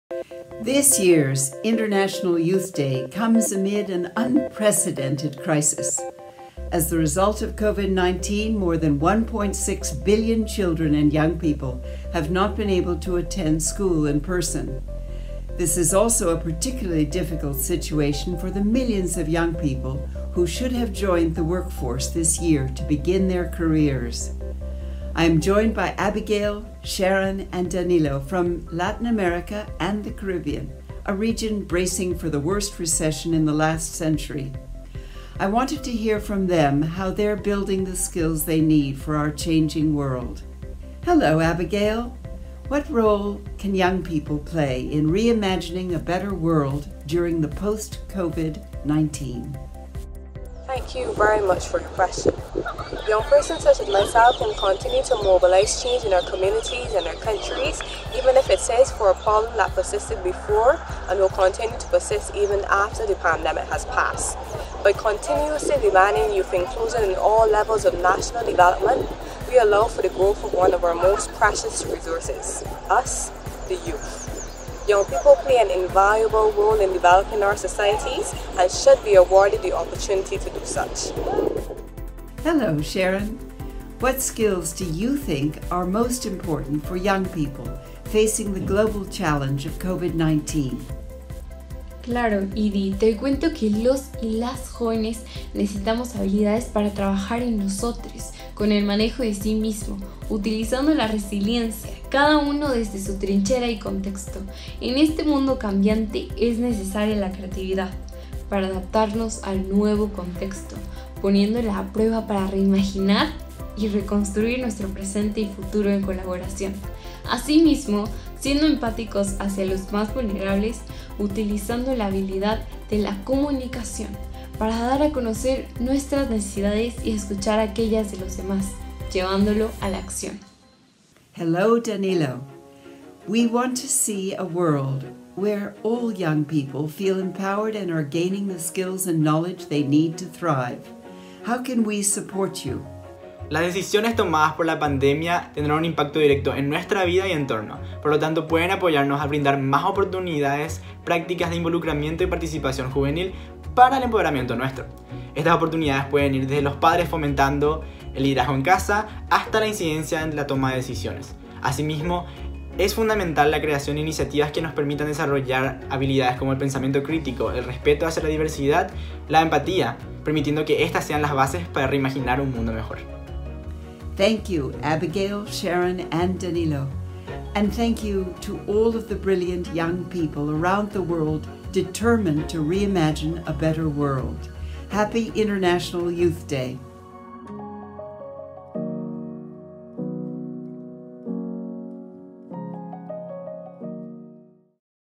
Día Internacional de la Juventud 2020
En este 2020, el Día Internacional de la Juventud se hace acompañar de una crisis provocada por la pandemia de la COVID-19. Como resultado del Coronavirus, más de 1.6 billones de niños, adolescentes y jóvenes no han podido asistir a la escuela presencialmente.

¿Cómo la juventud reimagina un nuevo mundo?